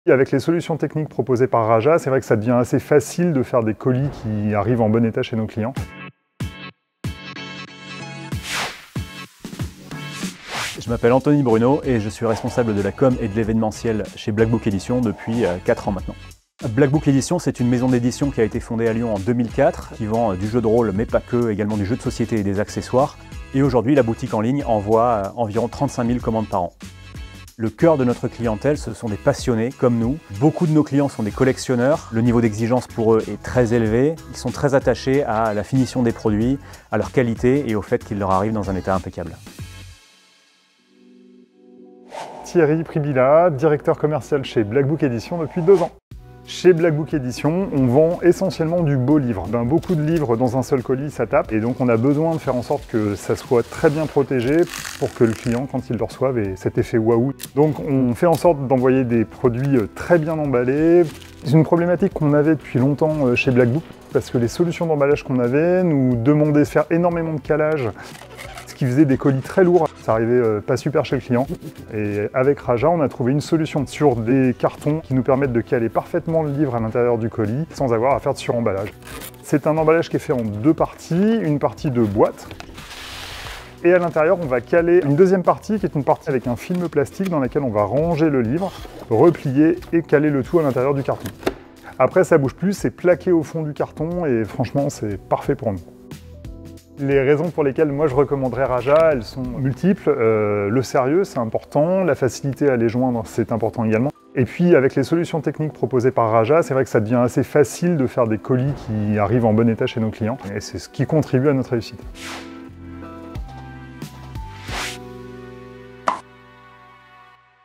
Interview Client RAJA - Black Book Éditions
Anthony Bruno, Responsable évènementiel et Thierry Przybyla, Directeur Commercial chez Black Book Éditions nous expliquent le partenariat entre RAJA et cette maison d'éditions spécialisée dans le jeu de rôle et les jeux de société.

Découvrez également, comment RAJA a accompagné Black Book Éditions pour améliorer son expérience client au travers de ses emballages.